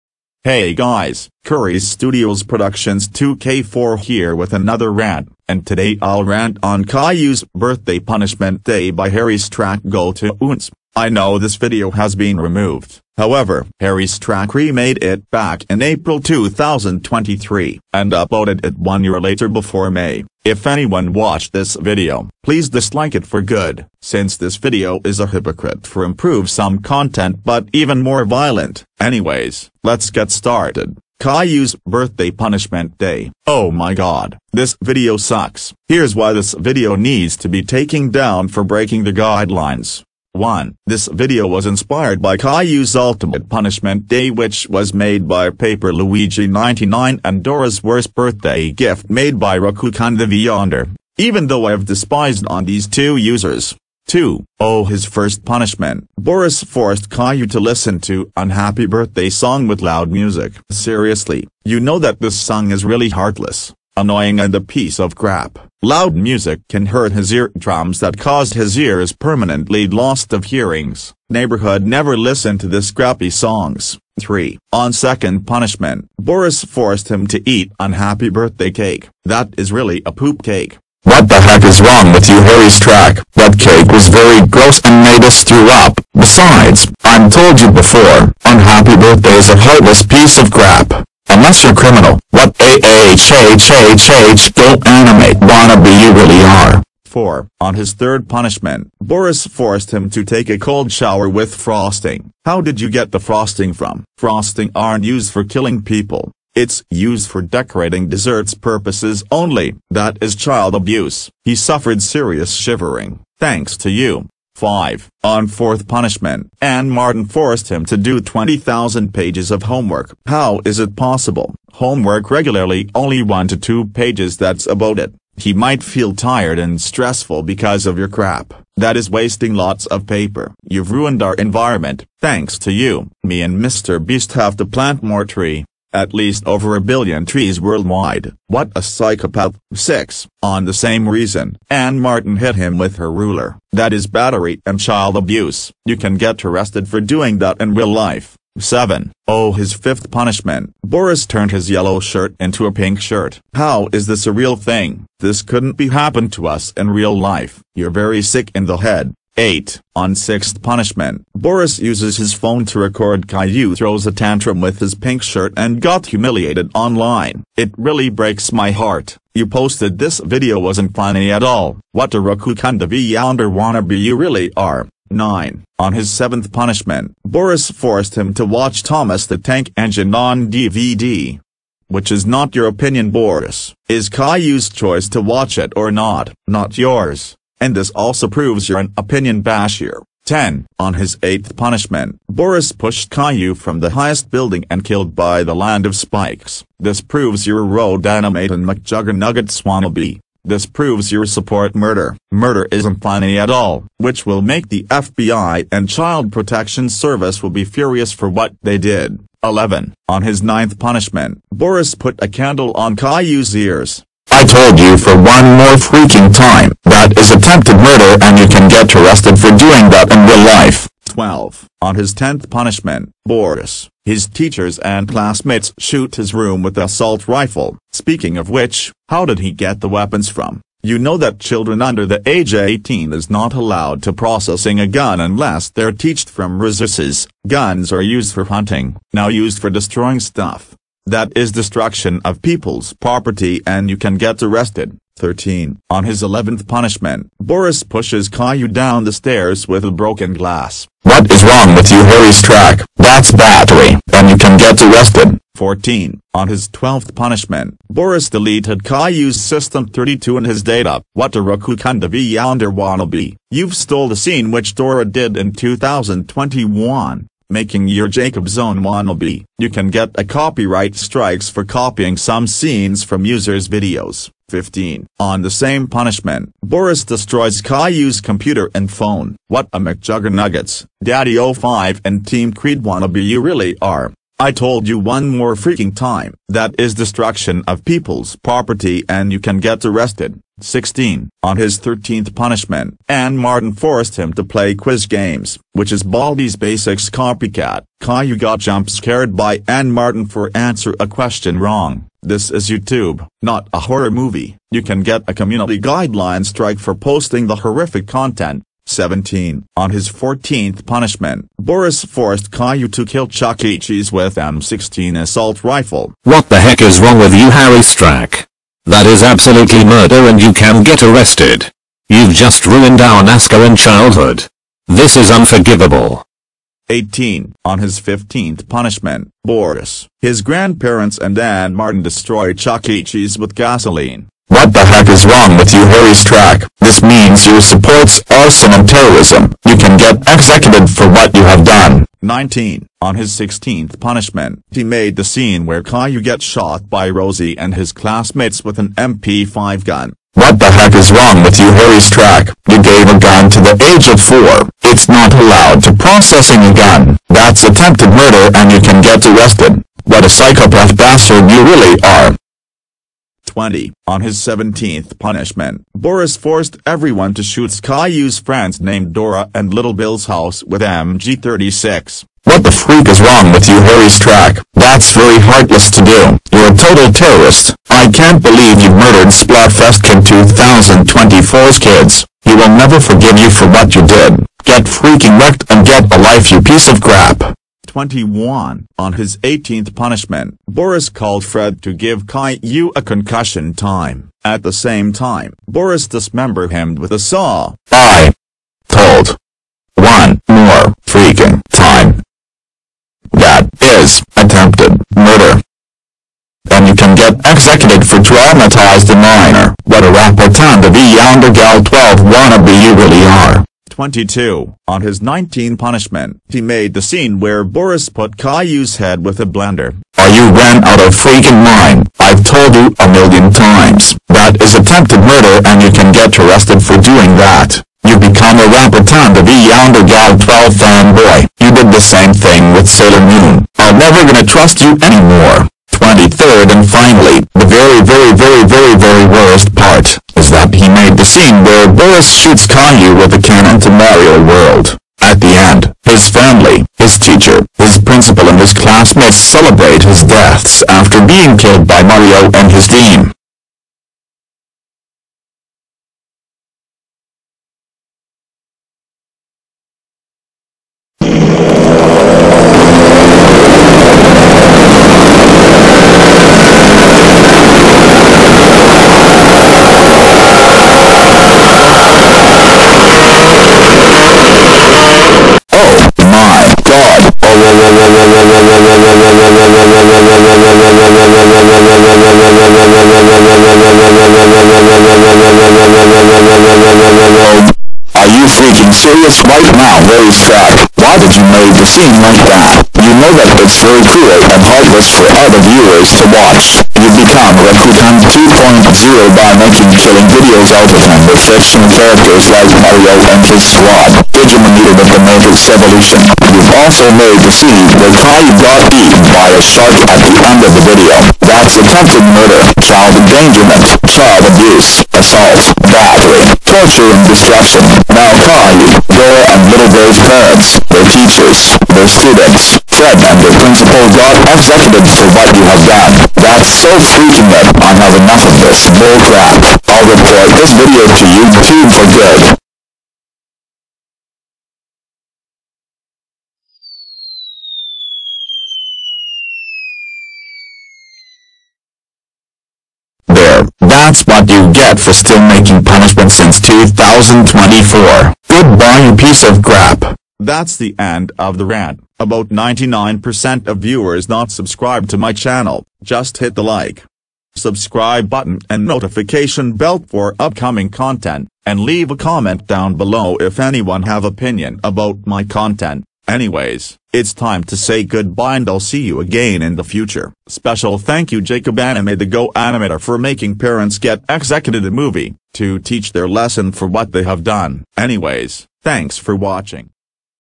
Official Rant 5: Caillou's Birthday Punishment Day made by Harry Strack (2024 Edition)
Warning: This video has some loud noises. Make sure children under 13 is not allowed to watch this video!

Harry Strack's Caillou's Birthday Punishment Day Video uploaded since 2022. Got deleted couple of months later. However. Harry Strack remakes it back in 2023 for improve some content and uploaded this year in April. Become one of the worst GoAnimate videos in 2024. Enjoy the rant and think about why this hypocrite videos needs to taking down for breaking the guidelines.

Must Watch:
Special Thanks to:
@animemangakun104 for a movie about Caillou's parents, his teacher, his principal and his classmates taught their lessons for what they have done.